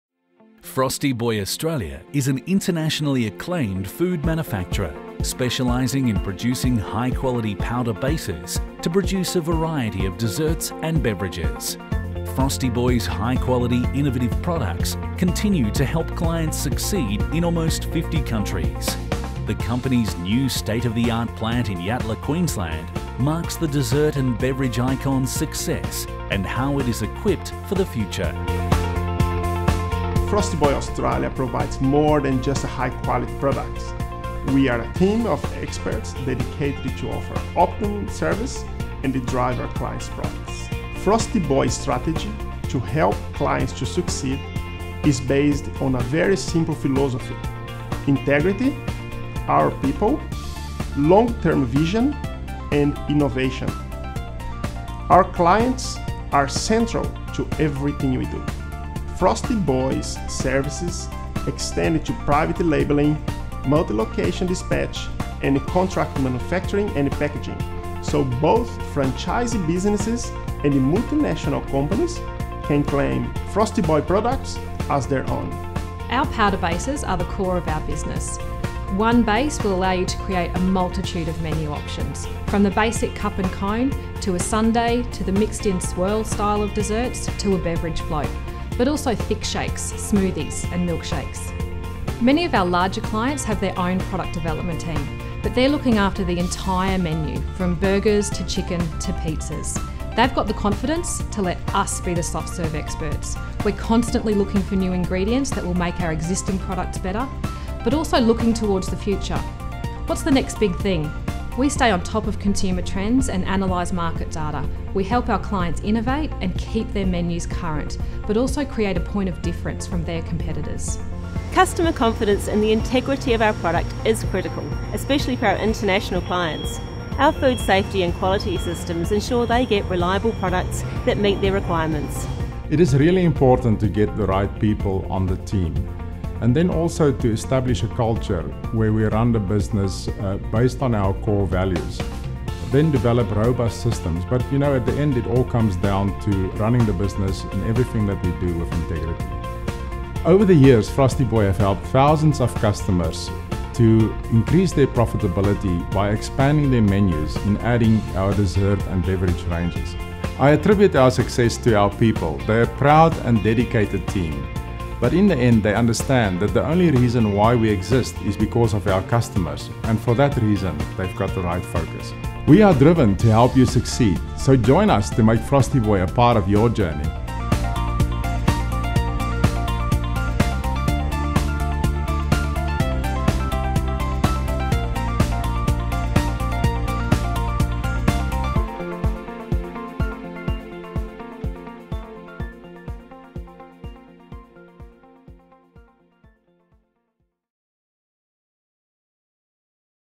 Frosty Boy Australia Corporate Video
Frosty Boy Australia is an internationally acclaimed food manufacturer specialising in producing high quality powder bases to produce a variety of desserts and beverages. Frosty Boy’s high quality, innovative products continue to help clients succeed in almost 50 countries. The company’s state-of-the-art plant in Queensland, Australia marks the dessert and beverage icon’s success and how it is equipped for the future